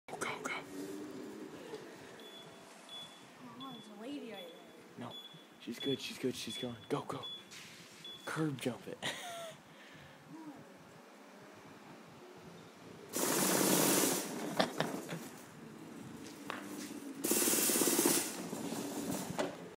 Draven steals handicap cart
Draven takes handicap cart for a test drive in the parking lot.